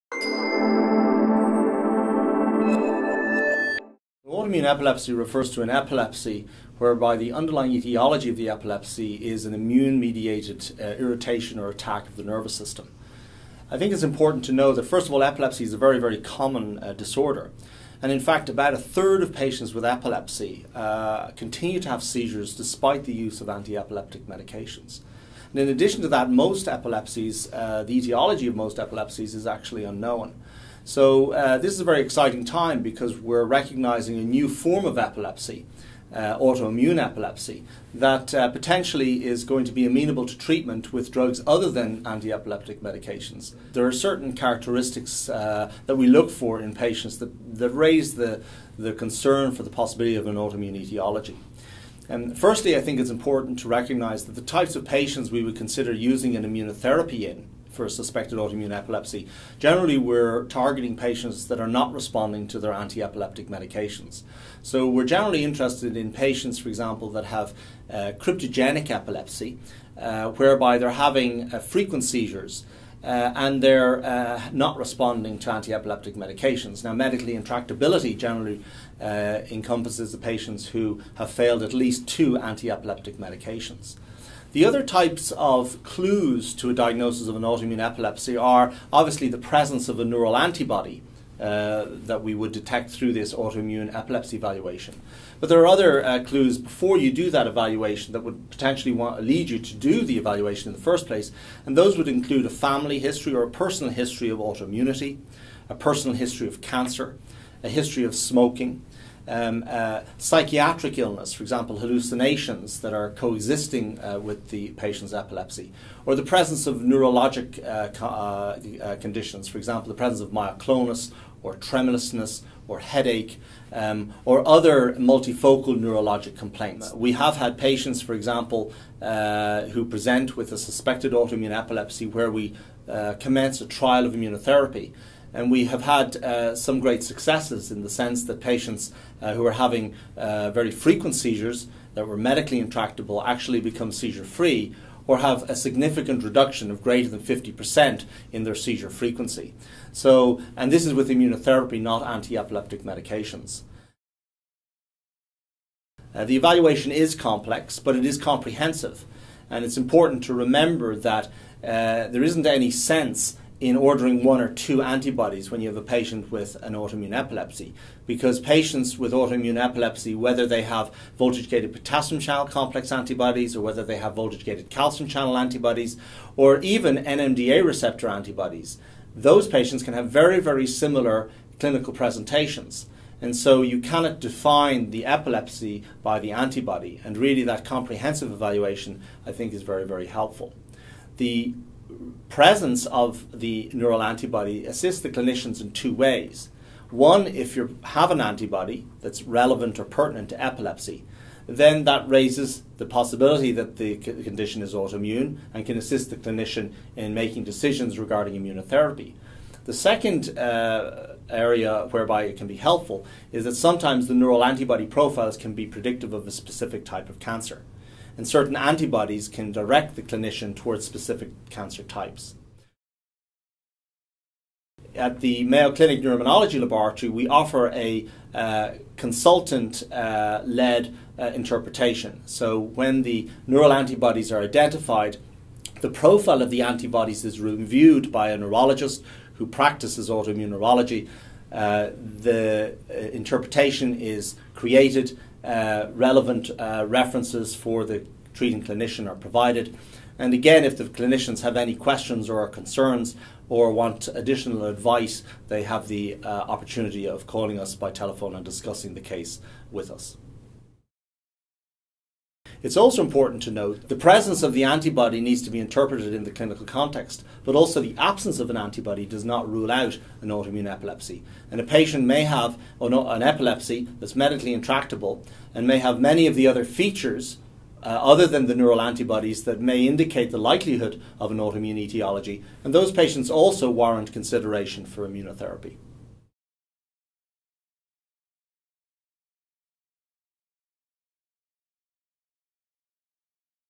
Epilepsy Autoimmune Evaluation
The new Epilepsy Autoimmune Evaluation is now available from Mayo Medical Laboratories. In this video, Sean Pittock, M.D., discusses what providers should know about autoimmune dementia, the testing approach, as well as interpretation, follow-up and additional considerations once the test has been ordered.